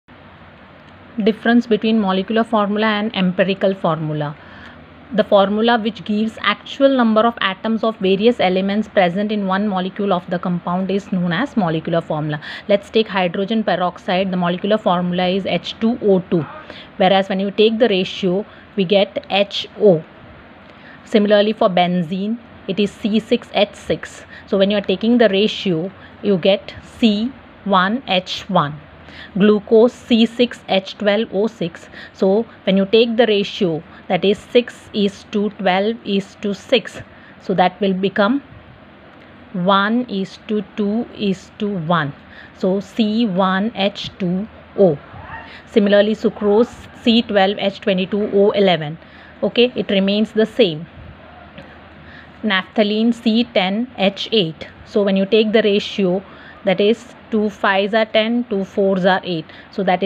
Shorts#Difference between molecular formula and empirical formula
What is the difference between molecular formula and empirical formula
Define molecular formula
Define empirical formula
Examples of molecular formula and empirical formula